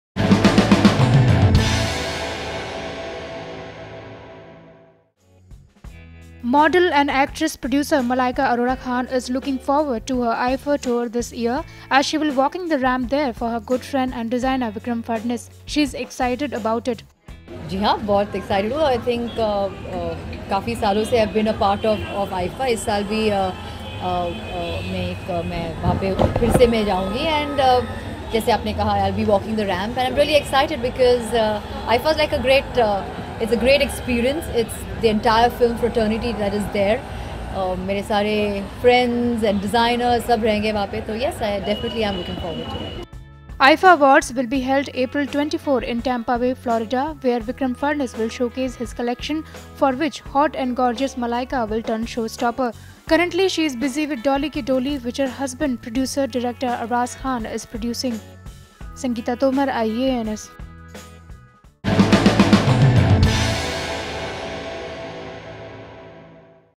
Malaika excited to walk the ramp at IIFA
Model and actress-producer Malaika Arora Khan is looking forward to her IIFA tour this year as she will be walking the ramp there for her good friend and designer Vikram Phadnis.